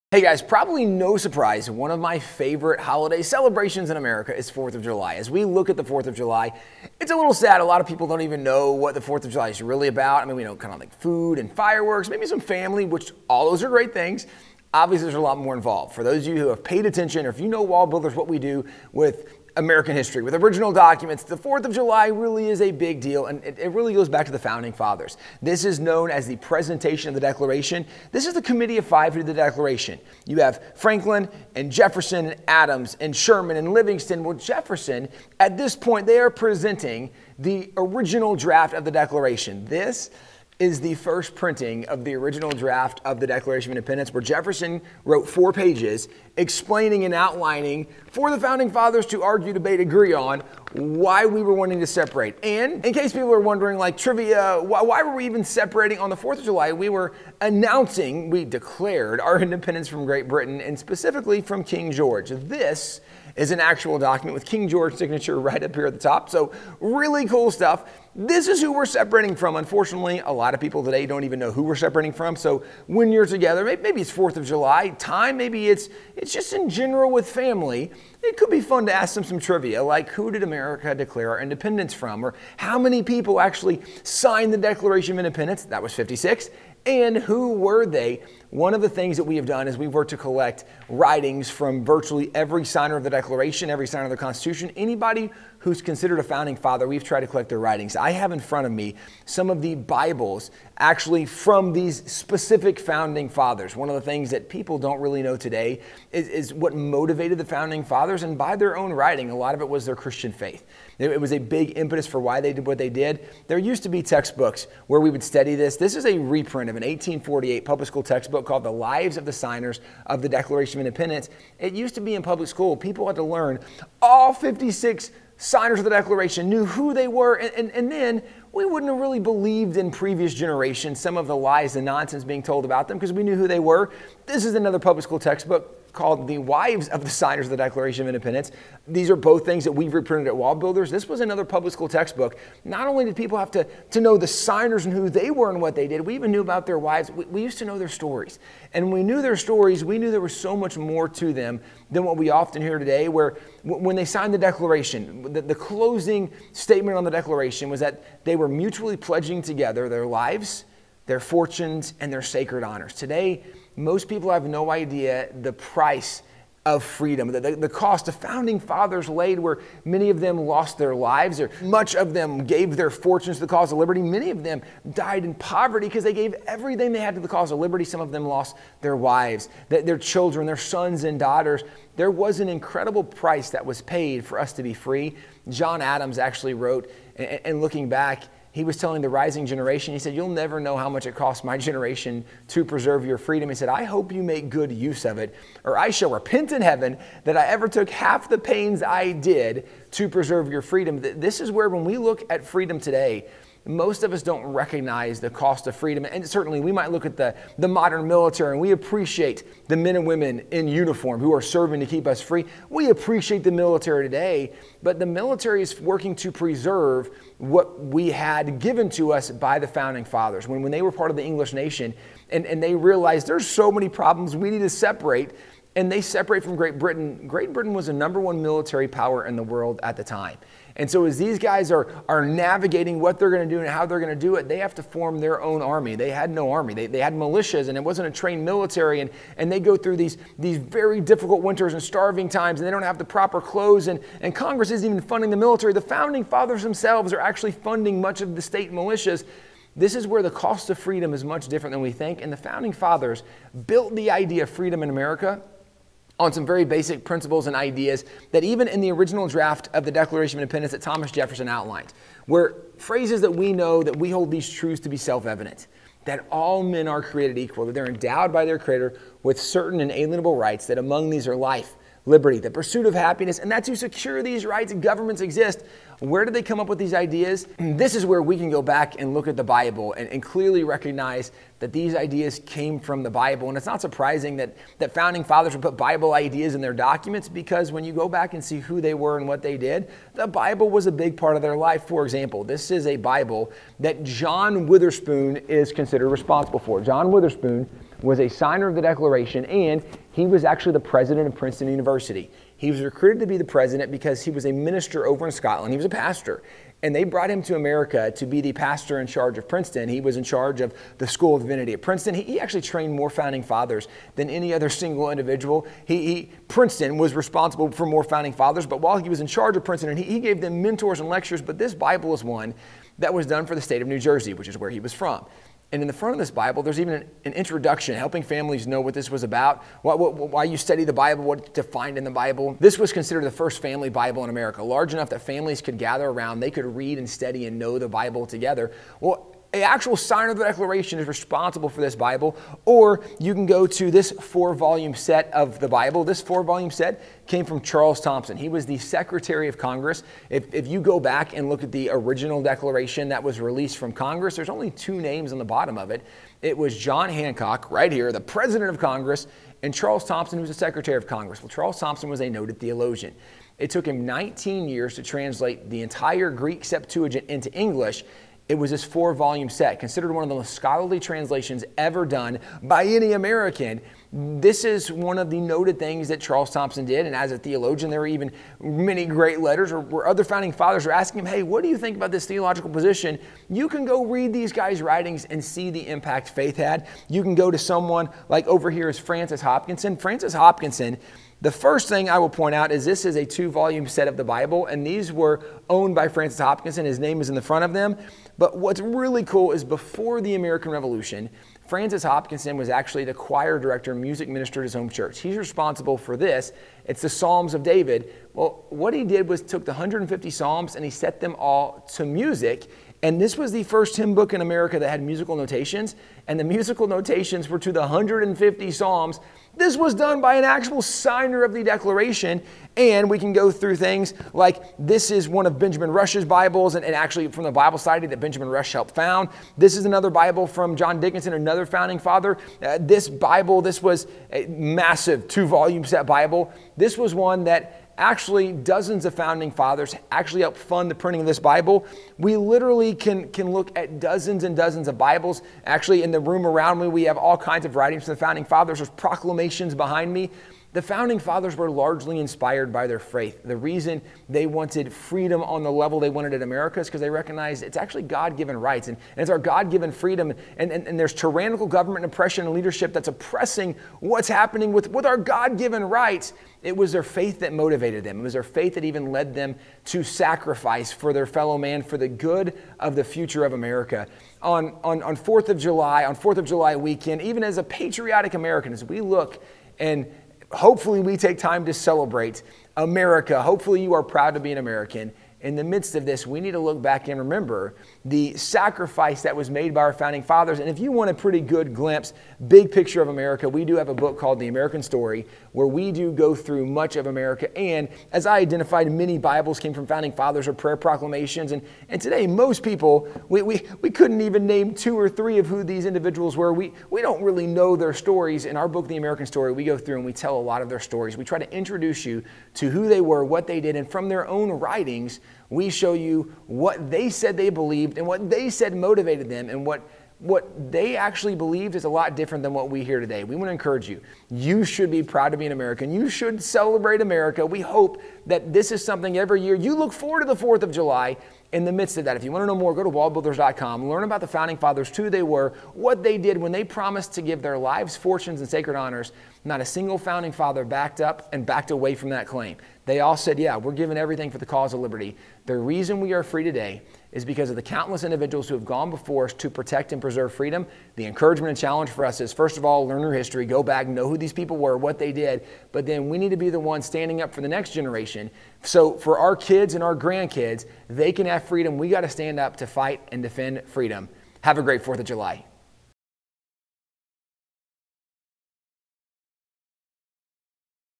Should we celebrate the Fourth of July?
Tim Barton, President of WallBuilders tells the truth about the Fourth of July.